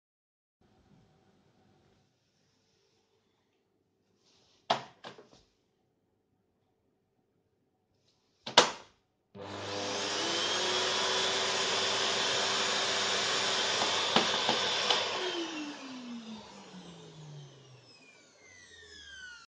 Kicking a Vacuum Cleaner by MisterKickSh*t
MisterKickShit kicks a vacuum cleaner